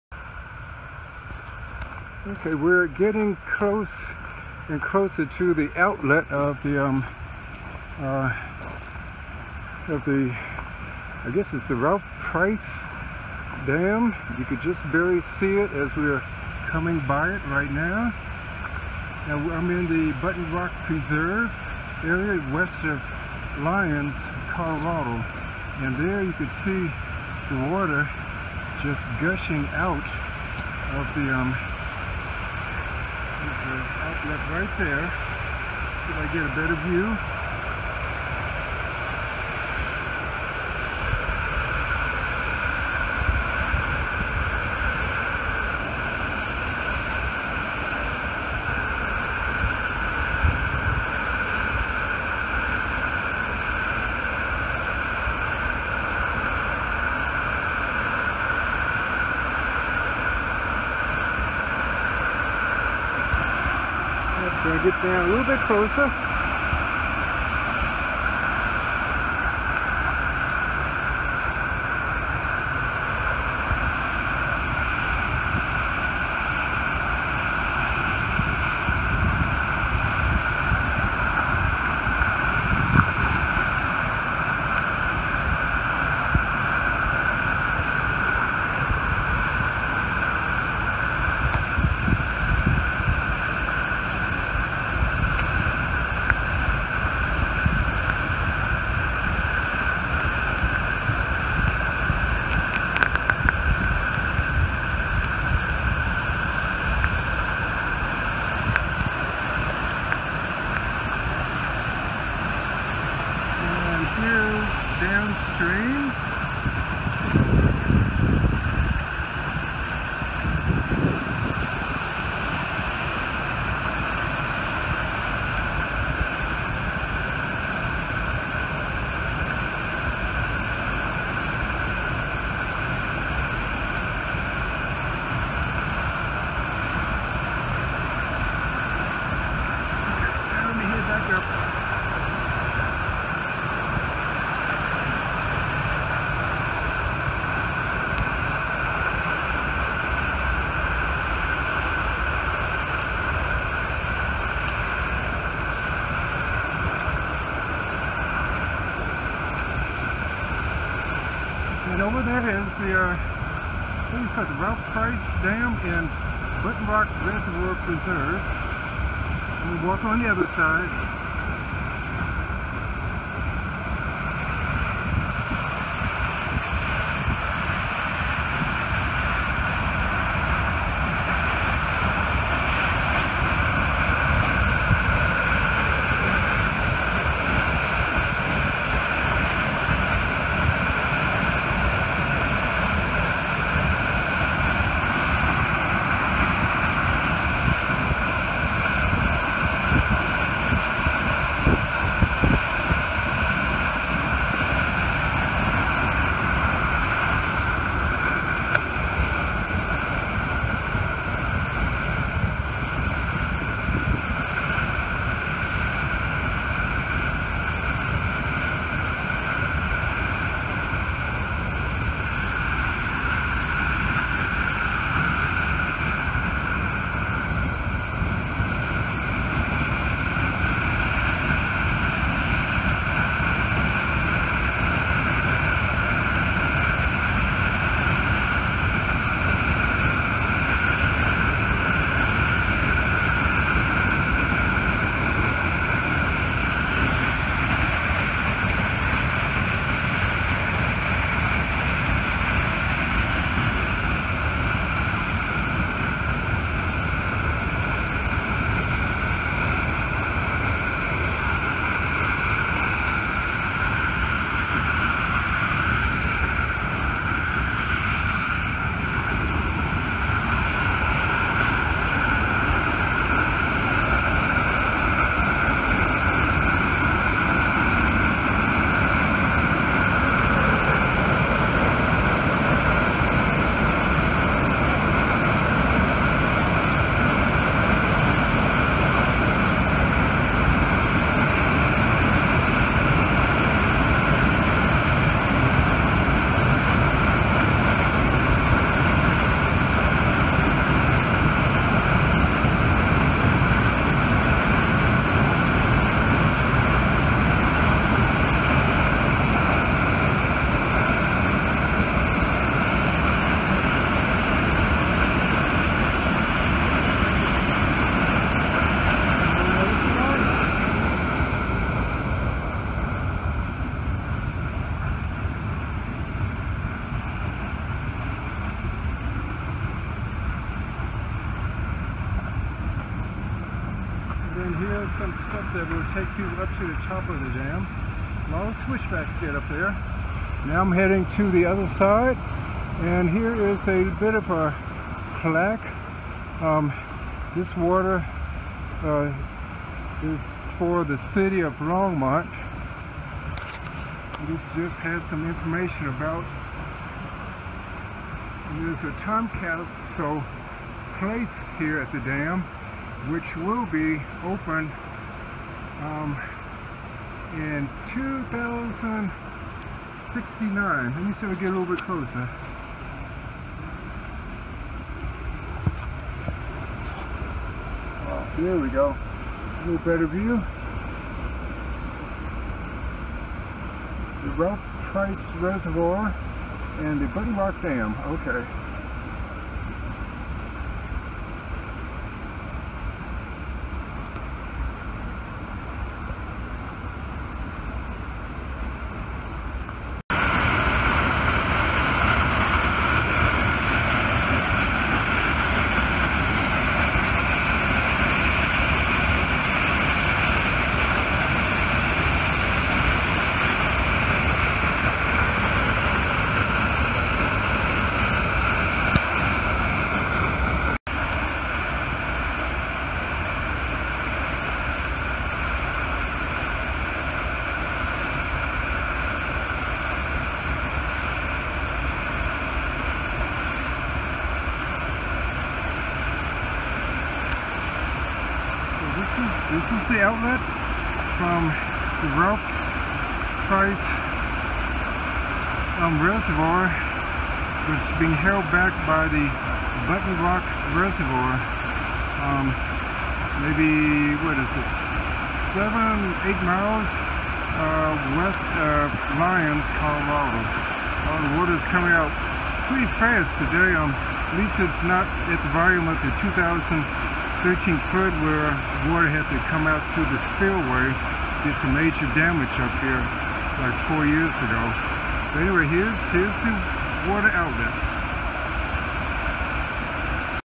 Button Rock Dam outlet
The easy hike of`4 miles round trip to see the outlet to the Button Rock Dam.

The hard, difficult hike of 4.7 miles to see the outlet to the Button Rock Dam.

On Saturday, May 6th, 2017, three members of the Beckwourth  Doers meetup group hiked in the Button Rock Preserve west of Lyons, Colorado. Two people in the group hiked 2 miles on the gradual ascending graded service road to the base to the Button Rock Dam and Ralph Rice Reservoir while I branched off about 0.75 miles from the parking lot, turning south on Sleepy Lion trail for the steep and difficult hike to rock outcroppings overlooking the Button Rock Dam in the valley below. The Sleepy Lion trail added 0.7 miles to reach the base of the Button Rock Dam and Ralph Price reservoir outlet.

Both hikes started from the parking at an elevation of 6,020' with the ending elevation of 6,253 feet at the Ralph Price reservoir outlet. The Sleepy Lion trail reaches the highest elevation of 6,625 feet with great views of the dam, the reservoir and views of Mt Meeker and Longs Peak to the west.

Watch the video to see the tremendous of water shooting over 75 feet from the outlet into the St. Vrain River.